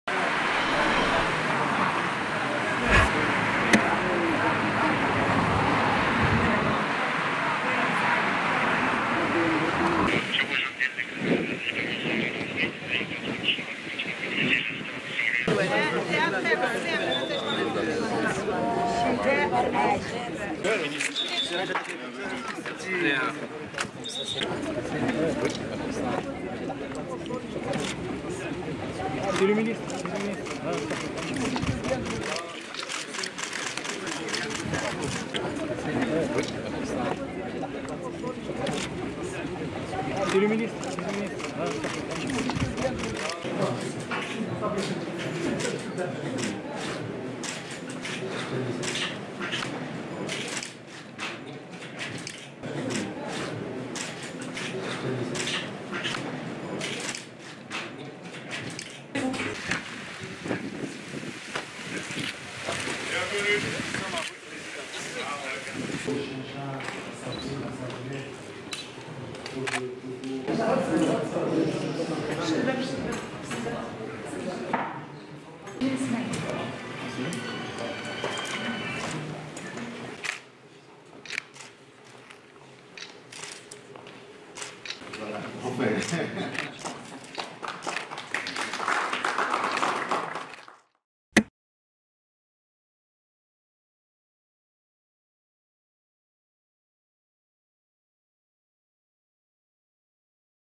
شاهد مراسم افتتاح جمهورية توغو لقنصلية عامة بمدينة الداخلة
افتتحت جمهورية توغو، صباح اليوم الخميس 21 يوليوز 2022، قنصلية عامة لها بمدينة الداخلة، وسط حضور وازن لمختلف الشخصيات السياسية والمدنية، إلى جانب ممثلي الهيئات الدبلوماسية بجهة الداخلة، ثاني حواضر الصحراء المغربية، في خطوة تؤكد الدعم الدولي المتزايد للمملكة في هذا الملف.